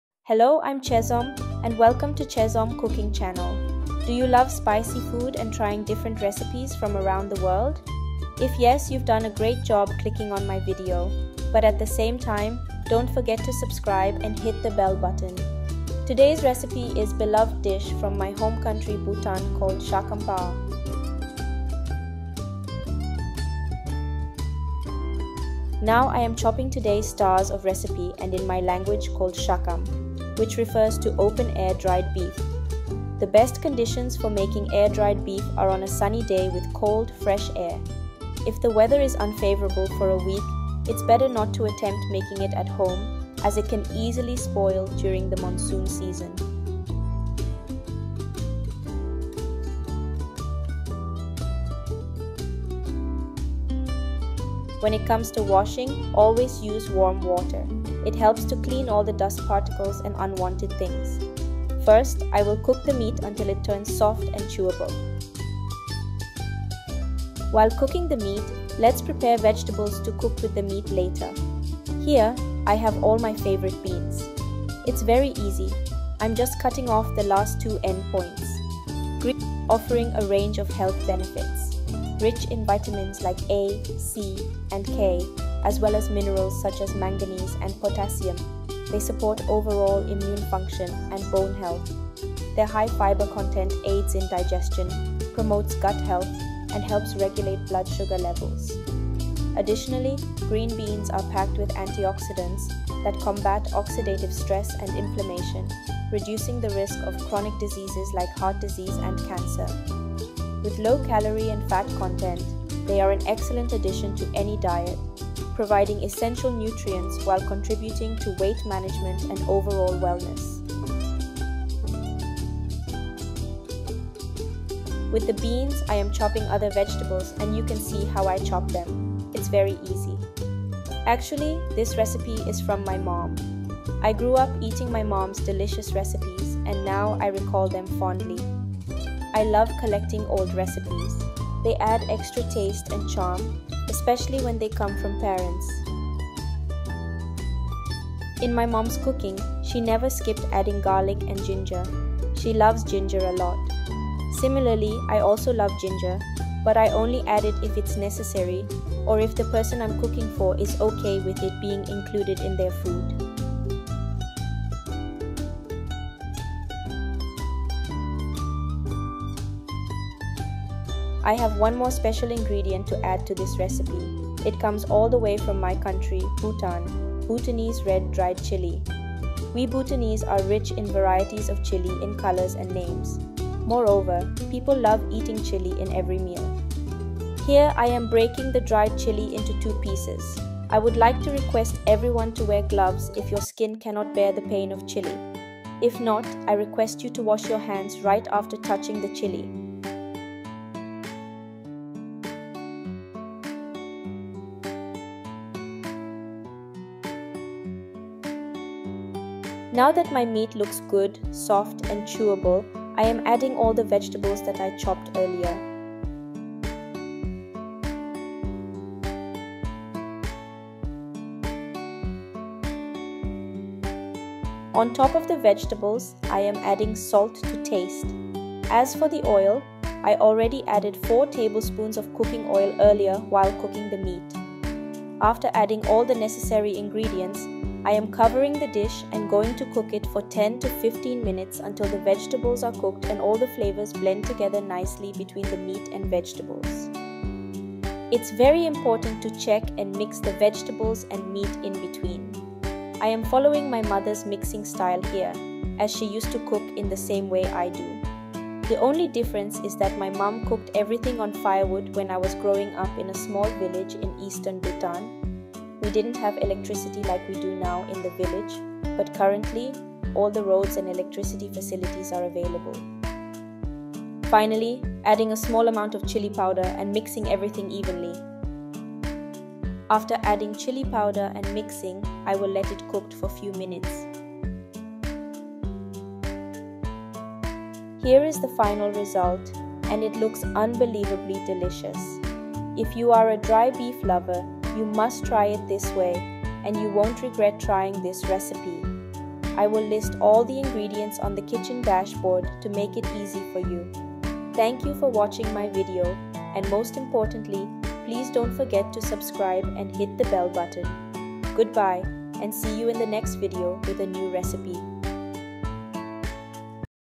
How to Make Authentic Bhutanese Norshakam Pa Recipe.
Air-dried beef: Rich in protein and nutrients, air-dried beef is a flavorful and traditional ingredient that adds depth to dishes like Norshakam Pa.
Red dried chili: Packed with capsaicin, red dried chili can boost metabolism, improve circulation, and add a spicy kick to the dish.
Green beans: A low-calorie vegetable, green beans are high in fiber, vitamins C and K, and minerals like manganese, making them great for digestion and overall health.
Onions: Onions contain antioxidants and compounds that may reduce inflammation and cholesterol levels, and they add a savory flavor to the dish.
Tomato: Rich in vitamins A and C, potassium, and lycopene, tomatoes contribute to heart health, skin health, and overall well-being while adding a tangy sweetness to the recipe.
Garlic: Garlic is known for its immune-boosting properties, as well as its ability to lower blood pressure and improve cholesterol levels, while adding a distinctive flavor to the dish.
Ginger: Ginger has anti-inflammatory and digestive benefits, aids in nausea relief, and adds a warm, spicy flavor to the dish.
Salt: While moderation is key, salt helps to enhance the flavors of the other ingredients and provides essential electrolytes for bodily functions.
Cooking oil: Provides healthy fats necessary for energy and nutrient absorption, and helps to cook the ingredients evenly.
Water: Essential for hydration and cooking, water helps to create a flavorful broth and ensures the ingredients are properly cooked.
Chili powder: Apart from adding heat, chili powder may boost metabolism and has anti-inflammatory properties, enhancing both flavor and health benefits of the dish.

Bhutanese dry beef recipes, such as Norshakam Pa, are quite famous within Bhutan and increasingly gaining recognition outside the country. While Bhutanese cuisine may not be as widely known globally as some other Asian cuisines, it holds a unique and distinct place due to its use of local ingredients, spices, and traditional cooking methods.

Dry beef dishes are popular in Bhutan due to their ability to preserve meat in a flavorful and sustainable way, which is particularly important in a mountainous region with limited access to fresh ingredients year-round. The process of air-drying beef has been a traditional method of preservation in Bhutan for generations, and dishes like Norshakam Pa showcase the rich flavors and cultural heritage of the country.

In recent years, with increased interest in global cuisine and culinary diversity, Bhutanese dishes, including dry beef recipes, have been gaining attention among food enthusiasts and travelers seeking authentic experiences. Additionally, efforts to promote Bhutanese cuisine through cultural exchange programs, food festivals, and social media have contributed to its growing popularity on the international stage.

Overall, while Bhutanese dry beef recipes may not be as universally recognized as some other cuisines, they hold a special place within Bhutanese culture and are increasingly appreciated by people around the world for their unique flavors and cultural significance.